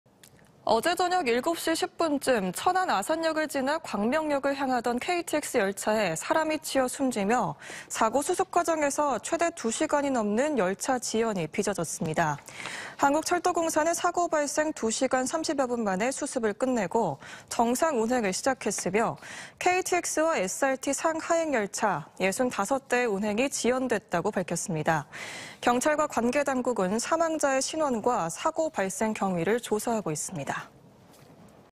천안아산역 인근 KTX에 치여 1명 사망..열차 2시간 넘게 지연｜ TJB 대전·세종·충남뉴스
어제 저녁
7시 10분쯤
천안아산역을 지나
광명역을 향하던
KTX 열차에
사람이 치여 숨지며
사고 수습과정에서
최대 2시간이 넘는
열차 지연이
빚어졌습니다.

한국철도공사는
사고 발생 2시간 30여분 만에
수습을 끝내고
정상 운행을 시작했으며
KTX와 SRT 상·하행 열차
65대 운행이 지연됐다고 밝혔습니다.

경찰과 관계당국은
사망자의 신원과 사고 발생 경위를